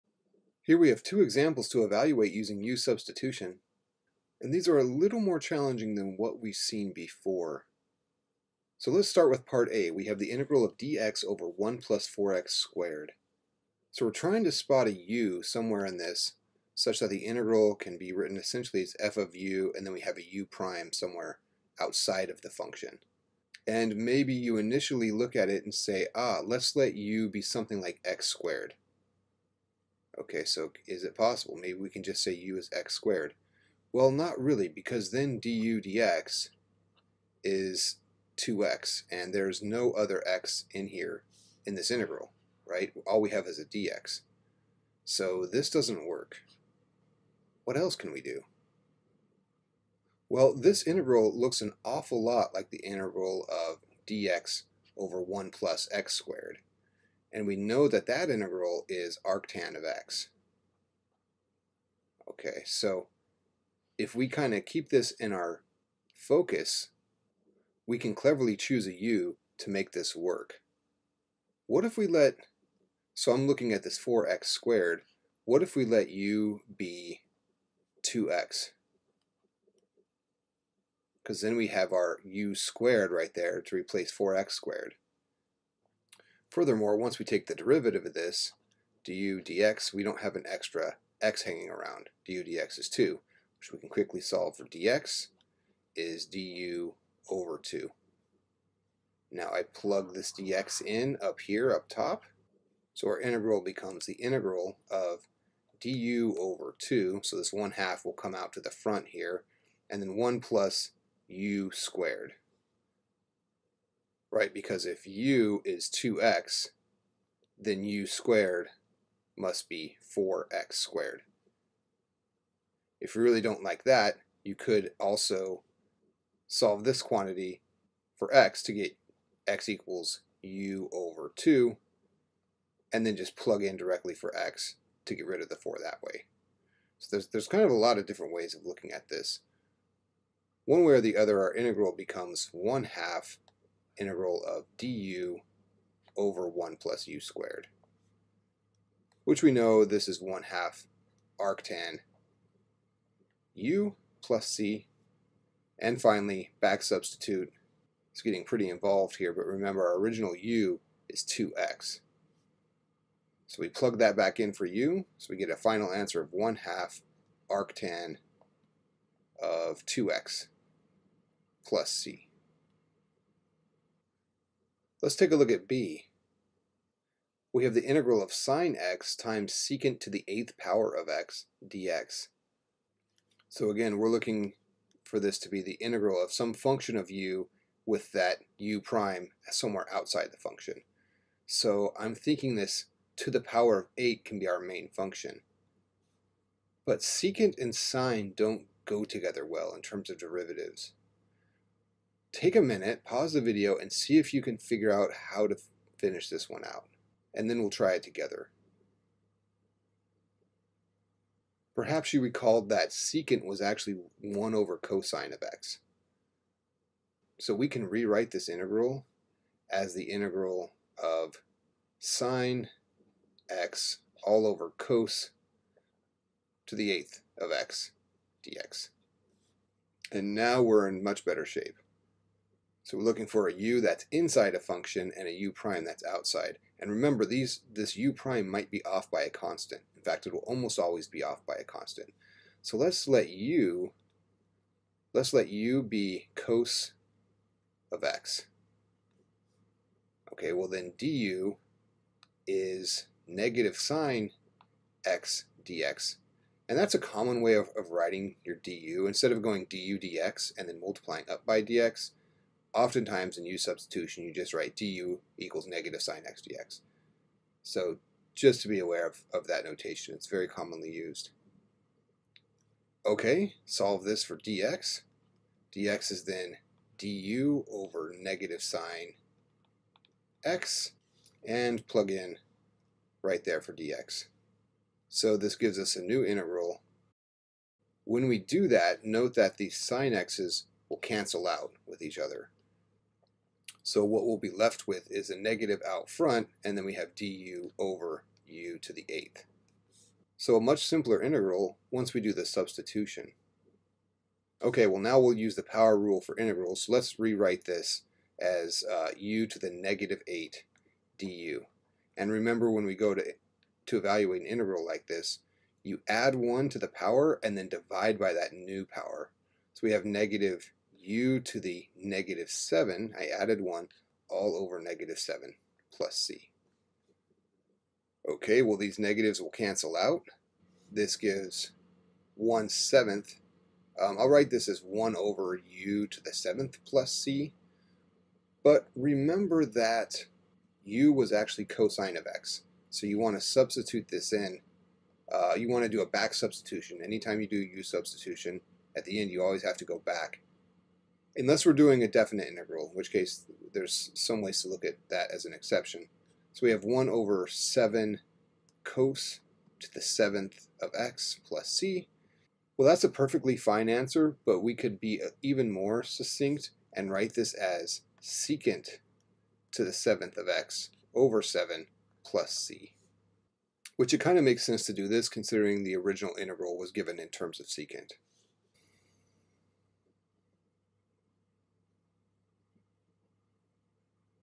Integrate Using u-Substitution – Two Slightly More Challenging Examples
This video gives two slightly more challenging examples of how to integrate using u-substitution. One involves a final result that includes arctan, and the other has us work with sec x.